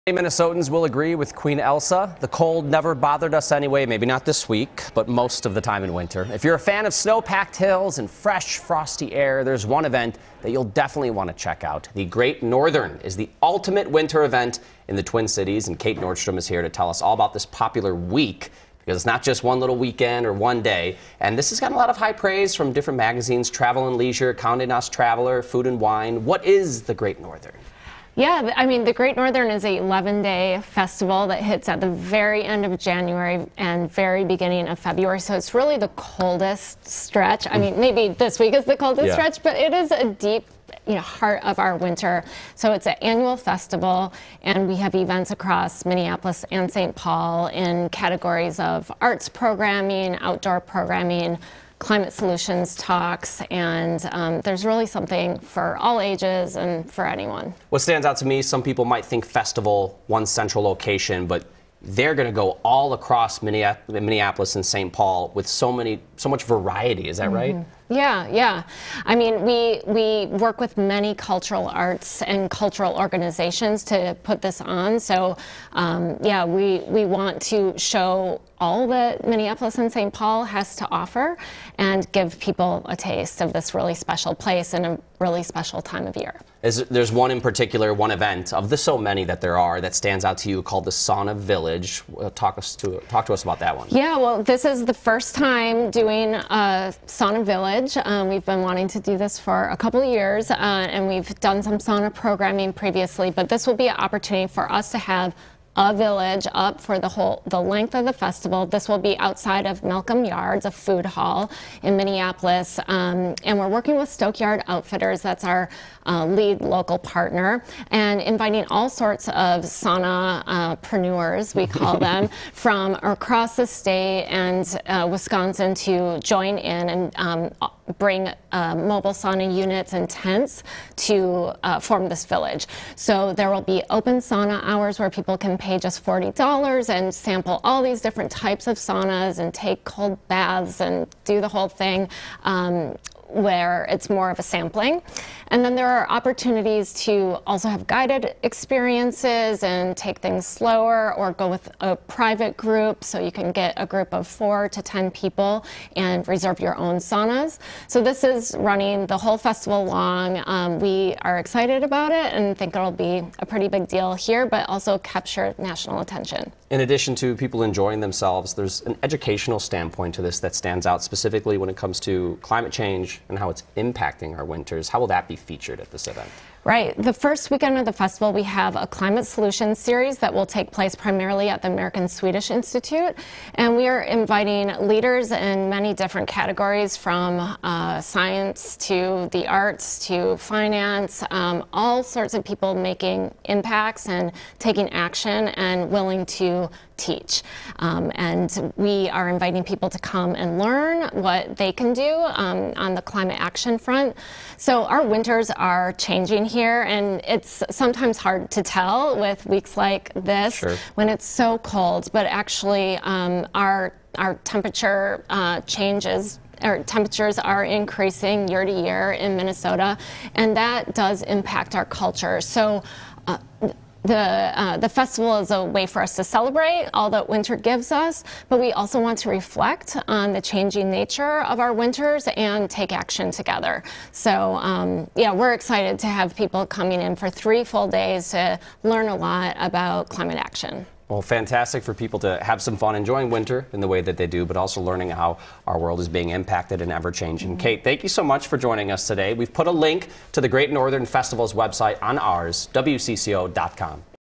The Great Northern coming to Twin Cities
Kate Nordstrum tells WCCO about The Great Northern, a festival celebrating winter in Minnesota.